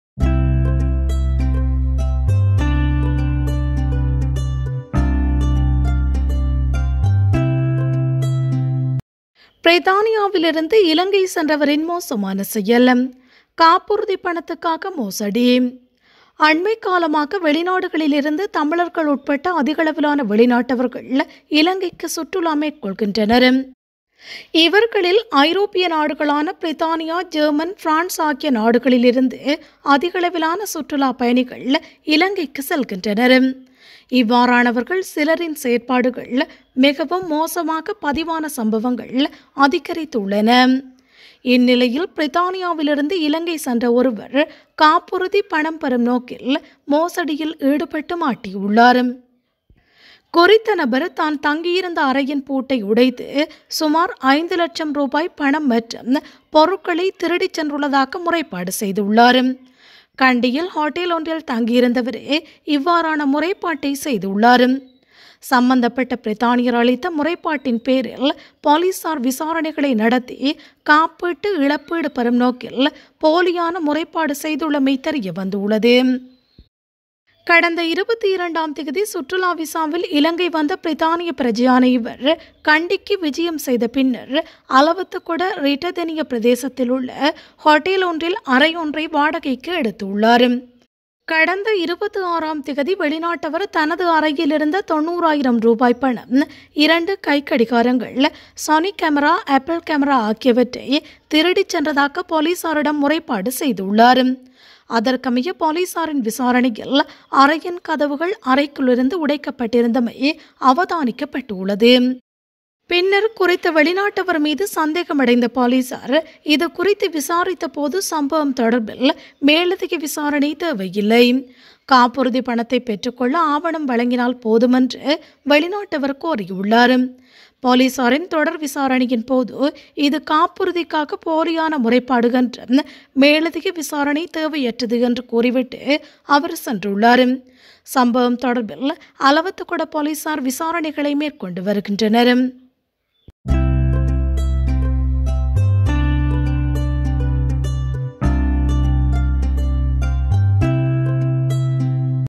பிரித்தானியாவில் காப்புறுதி பணம் பெறுவதற்காக இலங்கையில் நடந்த மோசடி அம்பலம்
அண்மைக்காலமாக வெளிநாடுகளில் இருந்து தமிழர்கள் உட்பட அதிகளவான வெளிநாட்டவர்கள் இலங்கைக்கு சுற்றுலா மேற்கொள்கின்றனர்.